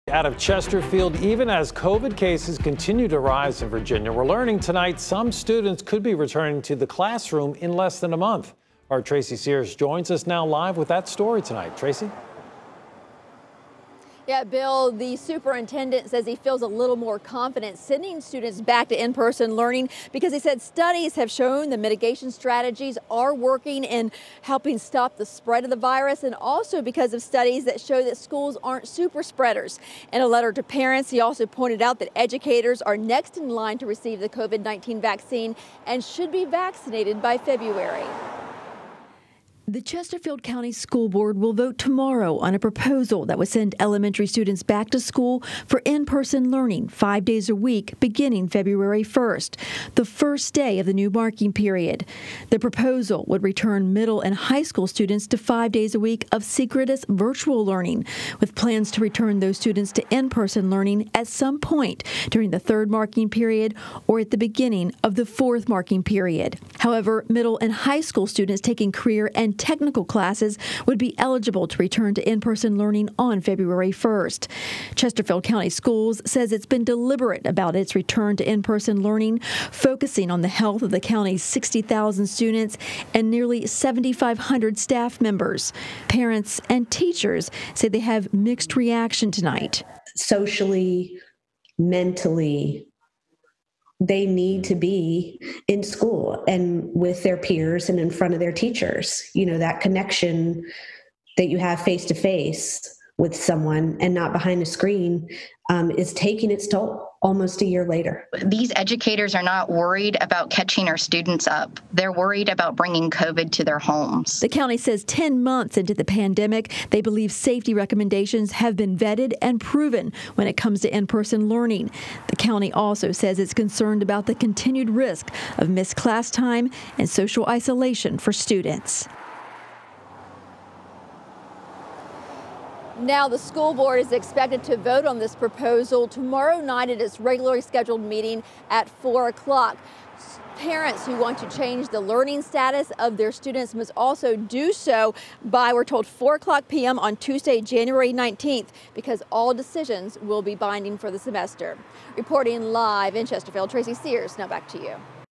Chesterfield Schools recommending return to in-person learning
In a school board meeting scheduled for Tuesday evening, Chesterfield County school leaders will recommend a new plan that would allow some students to return to in-person learning next month.